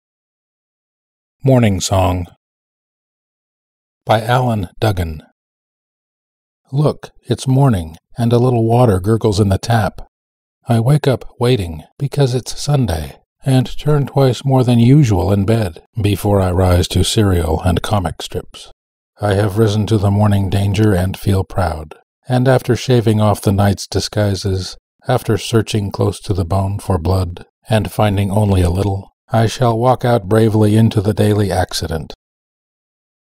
"Morning Song," by Alan Dugan
poem: Dugan--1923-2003; from COLLECTED POEMS (Yale, 1973, second printing). Image of razor from badgerblade.